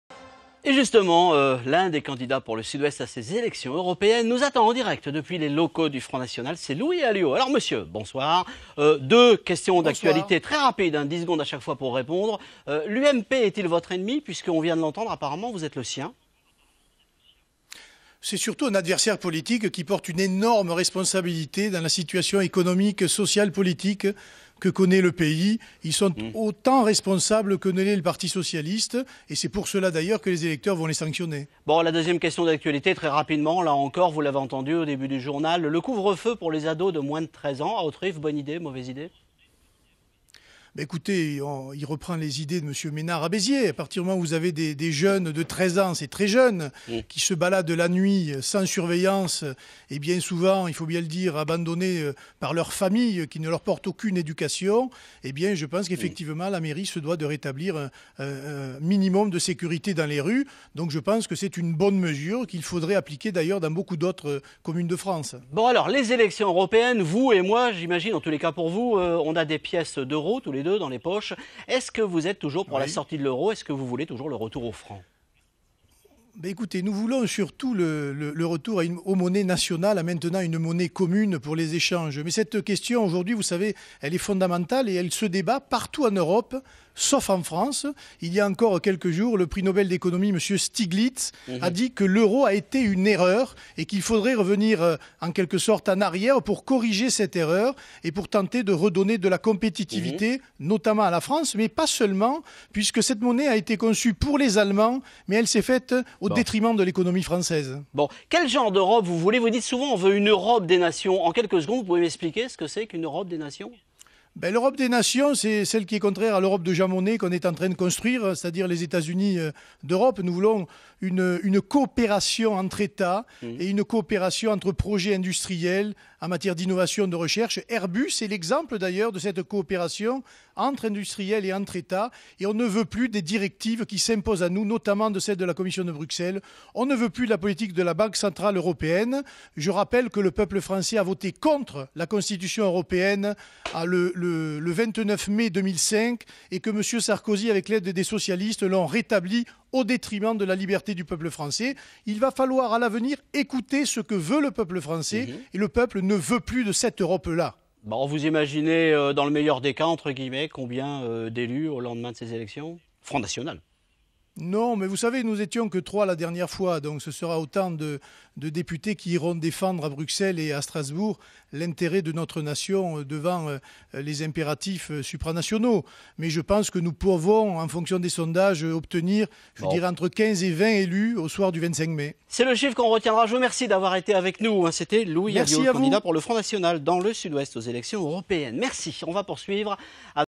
Louis Aliot sur France 3 Midi Pyrénes
Vendredi 16 Mai 2014, interview de Louis Aliot en direct des locaux du Front National de la Haute-Garonne à Toulouse sur France 3 Midi-Pyrénées.